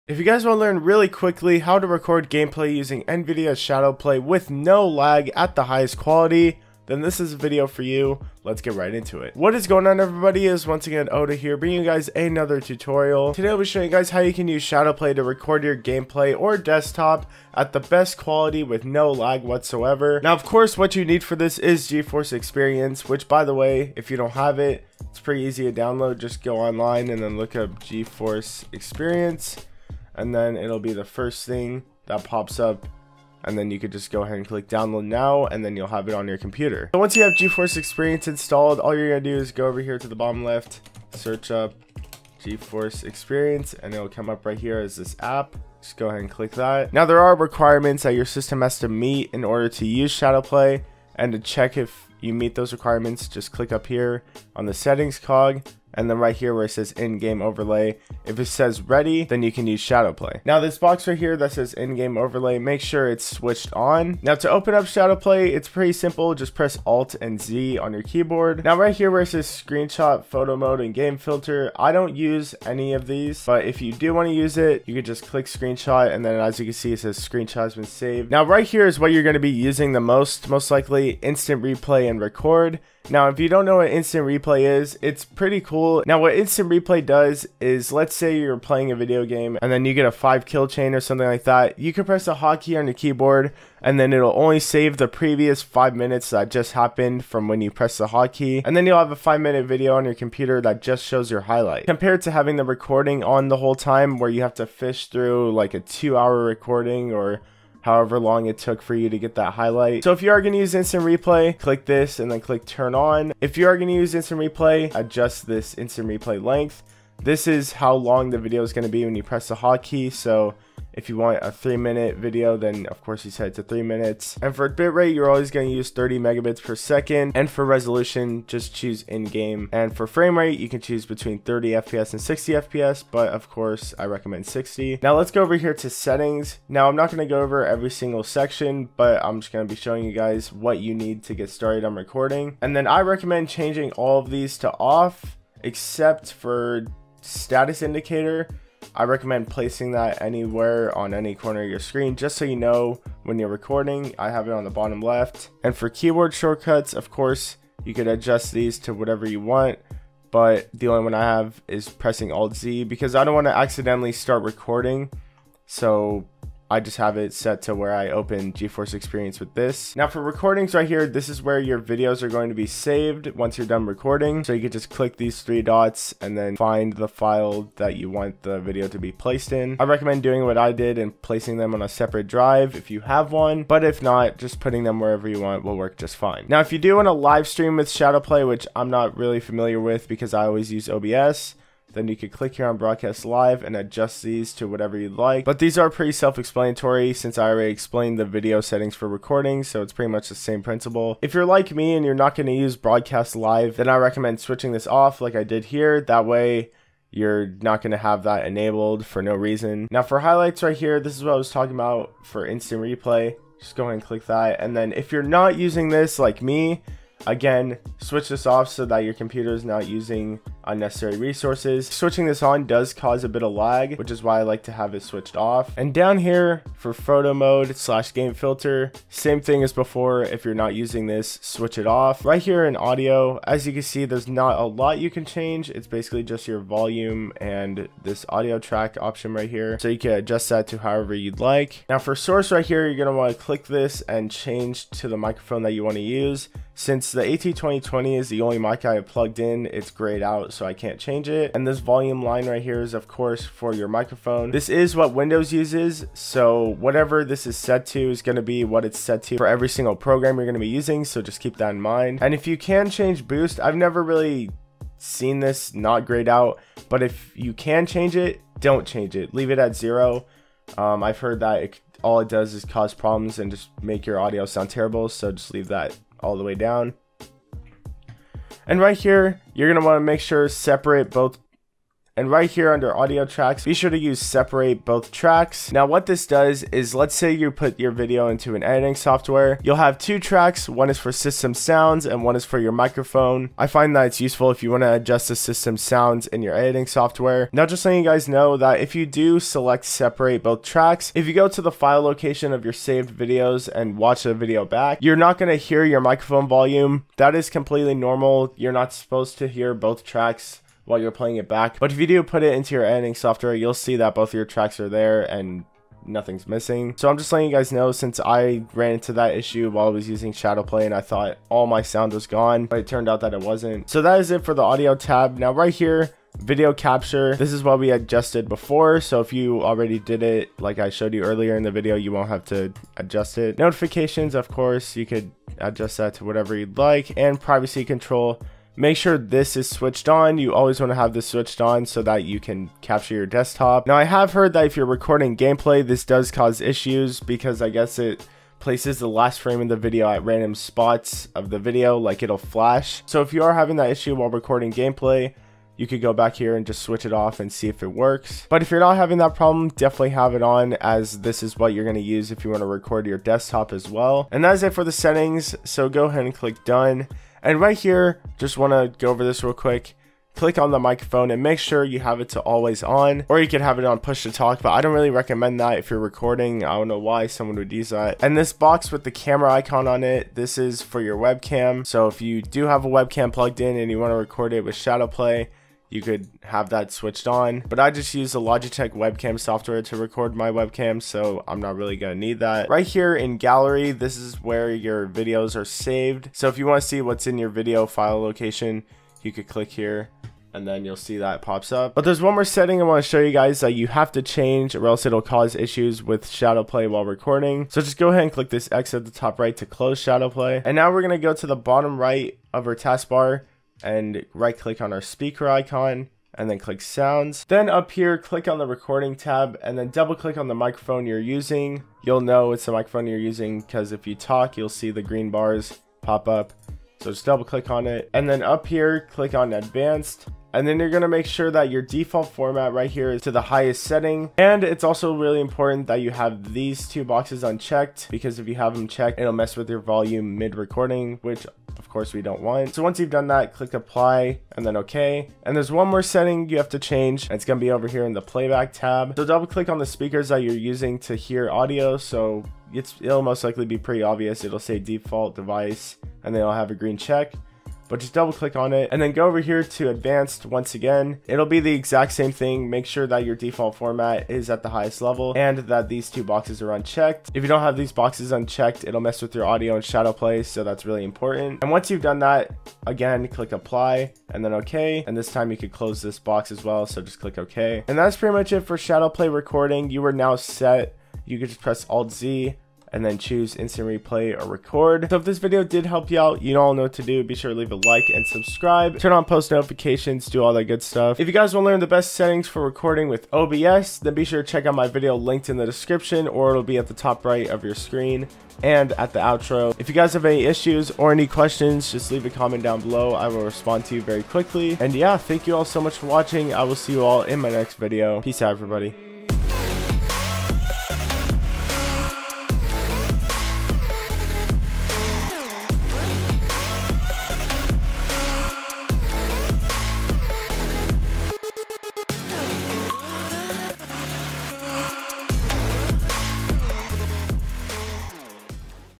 How to Record Gameplay Using NVIDIA Shadowplay: Lag-Free Recording
NVIDIA ShadowPlay is without a doubt the best option you have to record your desktop gameplay. I showcase the essential basic settings you should change before getting started so you can achieve maximum video quality without compromising in-game FPS.